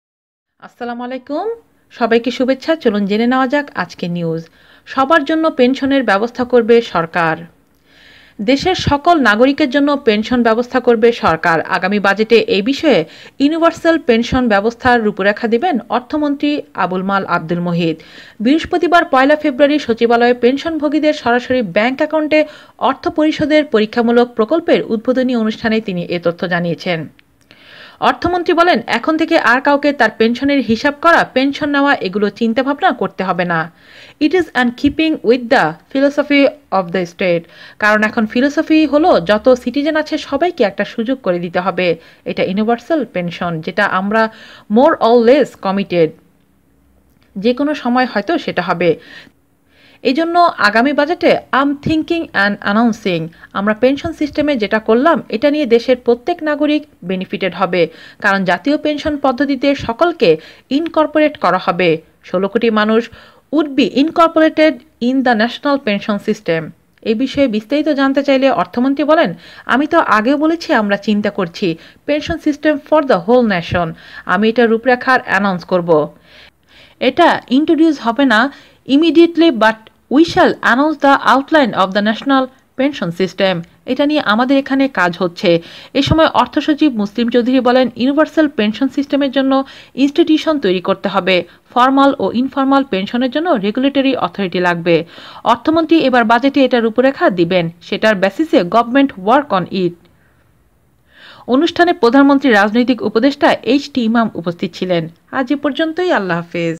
দেশের সকল নাগরিকের জন্য পেনশন ব্যবস্থা করবে সরকার: অর্থমন্ত্রী আবুল মাল আবদুল মুহিত!!
দেশের সকল নাগরিকের জন্য পেনশন ব্যবস্থা করবে সরকার। আগামী বাজেটে এ বিষয়ে ‘ইউনিভার্সল পেনশন’ ব্যবস্থার রূপরেখা দেবেন অর্থমন্ত্রী আবুল মাল আবদুল মুহিত।

বৃহস্পতিবার (১ ফেব্রুয়ারি) সচিবালয়ে পেনশনভোগীদের সরাসরি ব্যাংক অ্যাকাউন্টে অর্থ পরিশোধের পরীক্ষামূলক প্রকল্পের উদ্বোধনী অনুষ্ঠানে তিনি এ তথ্য জানিয়েছেন।

অর্থমন্ত্রী বলেন, ‘এখন থেকে আর কাউকে তার পেনশনের হিসাব করা, পেনশন নেয়া -এগুলো চিন্তা-ভাবনা করতে হবে না। ইট ইজ ইন কিপিং উইথ দ্য ফিলোসফি অব দ্য স্টেট। কারণ, এখন ফিলোসফি হলো যত সিটিজেন আছে সবাইকে একটা সুযোগ করে দিতে হবে। এটা ইউনিভার্সল পেনশন, যেটা আমরা মোর অল লেস কমিটেড, যে কোনো সময় হয়তো সেটা হবে।’

তিনি বলেন, ‘এ জন্য আগামী বাজেটে আই অ্যাম থিঙ্কিং অ্যান্ড অ্যানাউন্সিং। আমরা পেনশন সিস্টেমের যেটা করলাম, এটা নিয়ে দেশের প্রত্যেক নাগরিক বেনিফিটেড হবে। কারণ জাতীয় পেনশন পদ্ধতিতে সকলকে ইনকর্পোরেট করা হবে। ১৬ কোটি মানুষ উড বি ইনকর্পোরেটেড ইন দ্য ন্যাশনাল পেনশন সিস্টেম।’